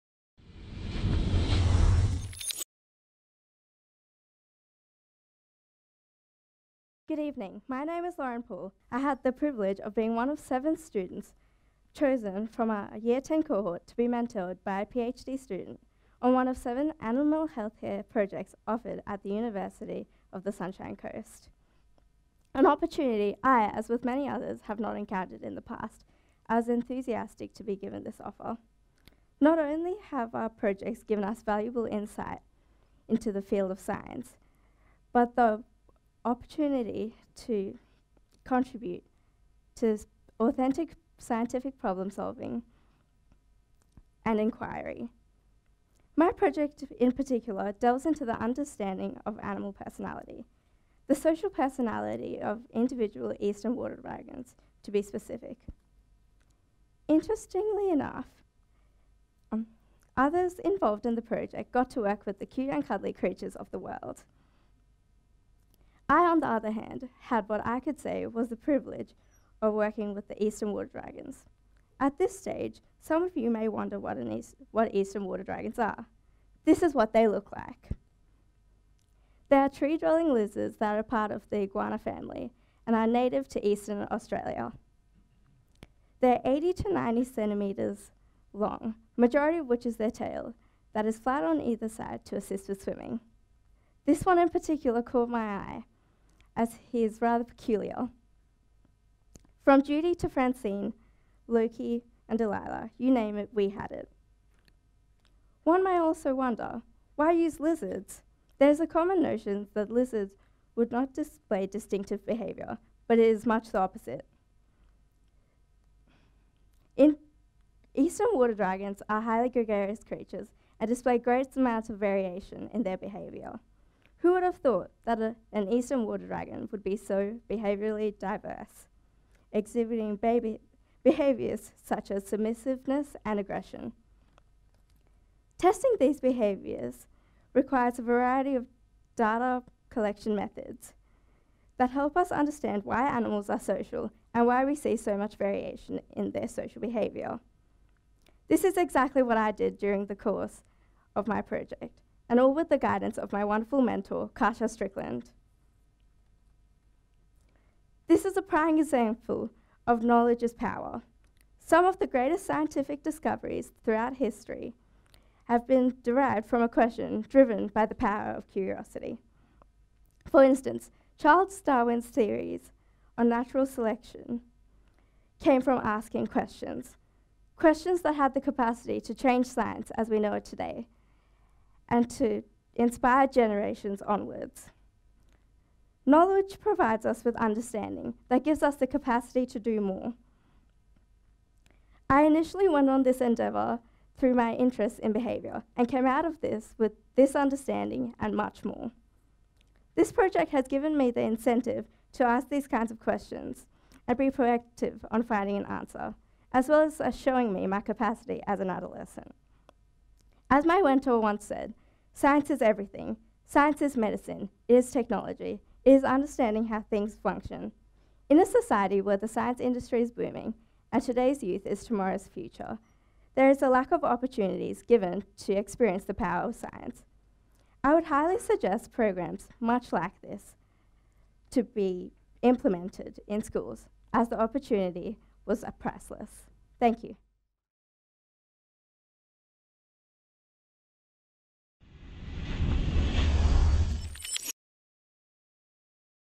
Potential In The Youth | Lauren Paul | TEDxGlasshouseChristianCollege
Editor: Jonty Brown

Organiser: Bert Kasselman

Location: Glasshouse Christian College

Lauren is a driven and inquisitive young woman. Born in Sri Lanka, she moved to Australia at the age of 7. Since then Lauren has been striving for the best. This project with the University of the Sunshine Coast has given her great amounts of insight into the realm of behaviour. Lauren aspires to be a psychologist in the future and has gained great amounts of insight into the science of behaviour, a key element in this particular field.